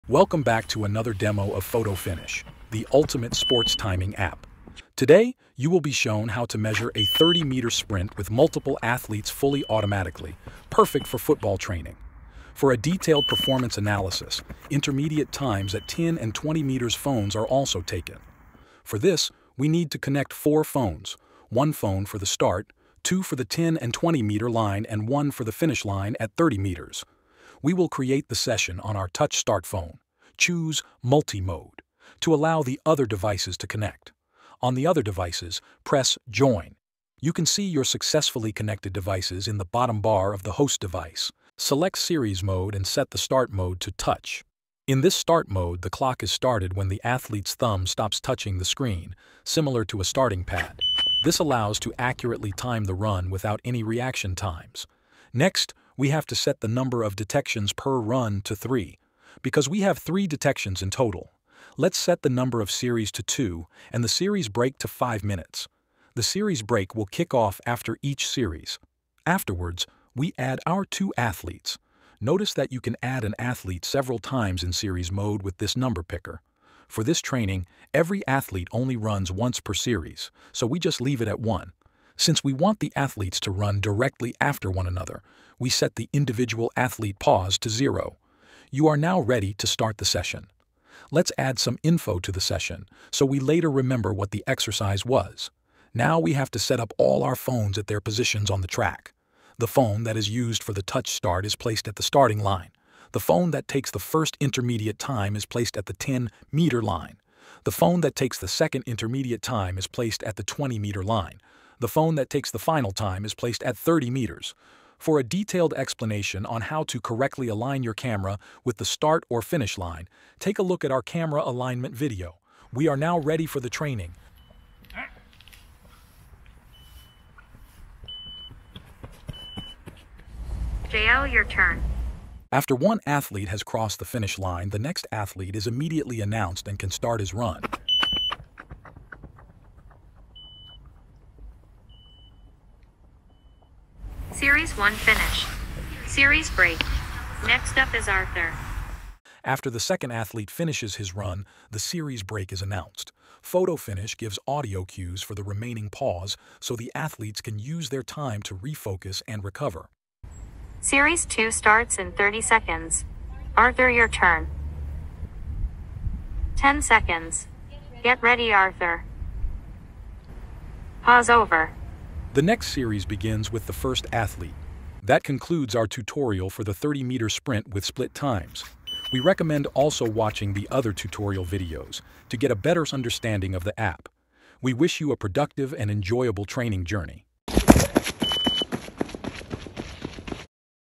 Photo Finish Tutorial - Touch Start with Split Times
In this video, you will learn how to use the series mode in combination with voice output for a detailed, automated performance measurement.

Voiceover was created with Elevenlabs.io.